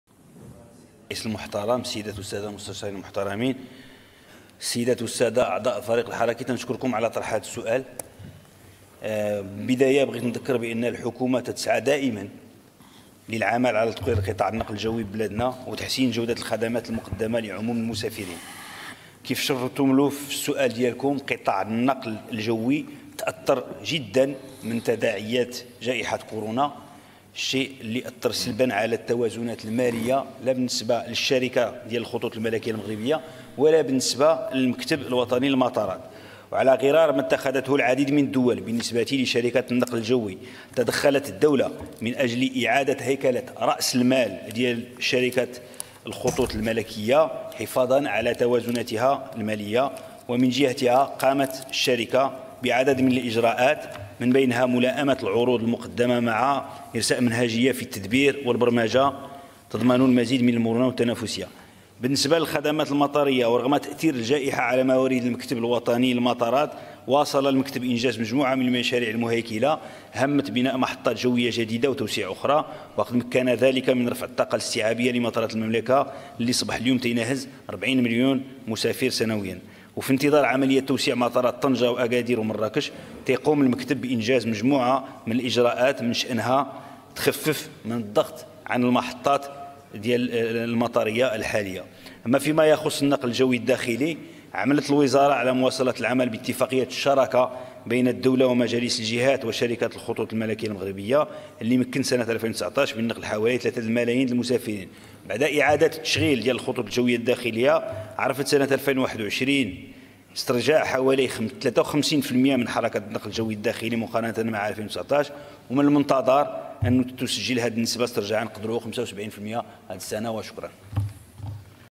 الإجراءات التي تقوم بها الوزارة لتطوير قطاع النقل الجوي وتحسين تنافسيته
جواب السيد محمد عبد الجليل، وزير النقل واللوجيستيك، خلال جلسة يوم الثلاثاء 07 يونيو 2022 بمجلس المستشارين، حول الإجراءات التي تقوم بها الوزارة من أجل تطوير قطاع النقل الجوي وتحسين تنافسيته